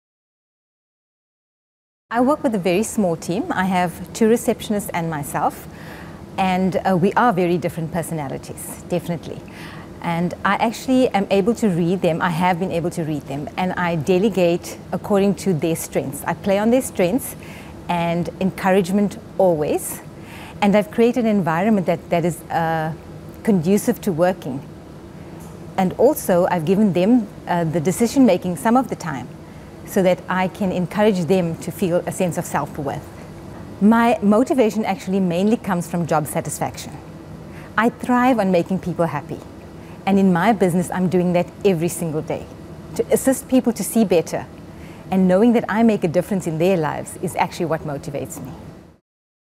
BWA RBAA 2013 - Anusha Naidu
Anusha Naidu was a finalist in the Businesswomens Association of South Africa's Regional Business Achievers Awards in the Professional Category.
This was the video screened at the awards gala.